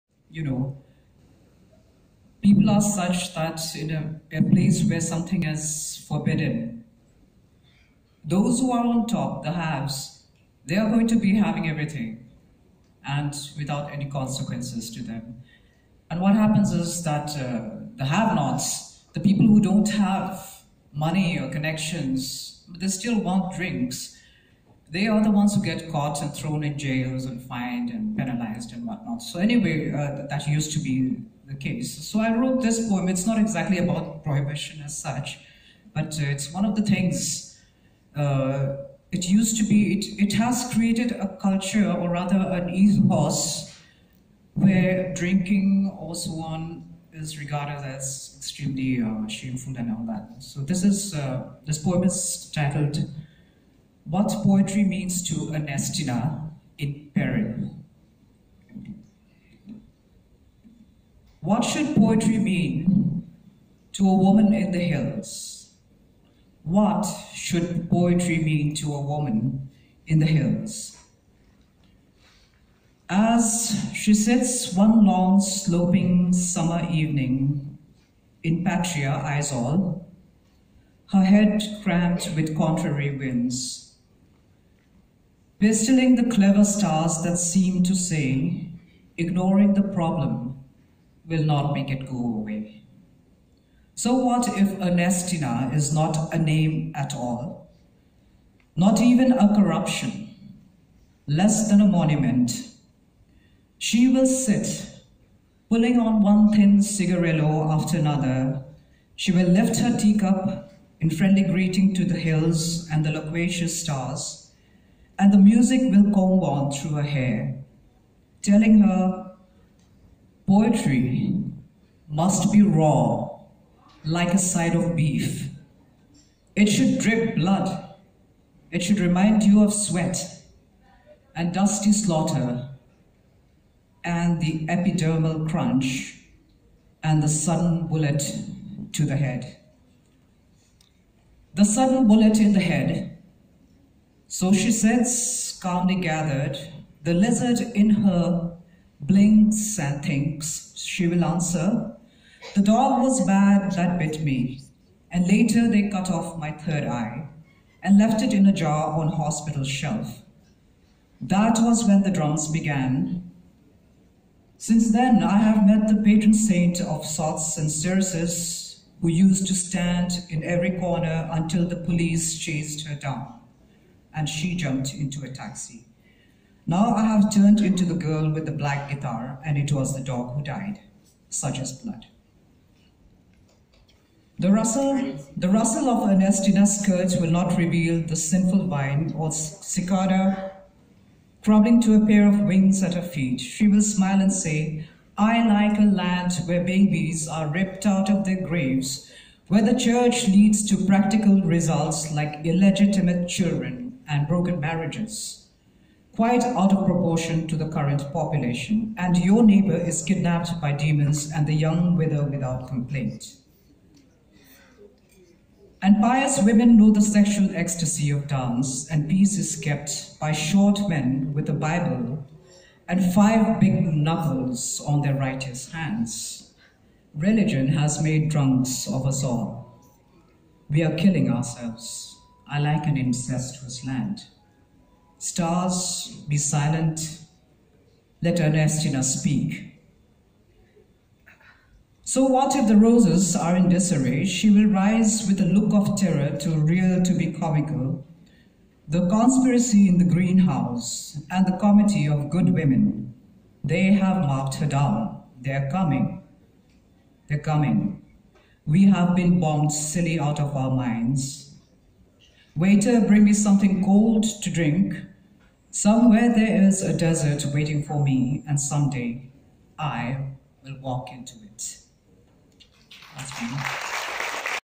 Mona Zote
Mona Zote during the Bengaluru Poetry Festival on 5 August 2018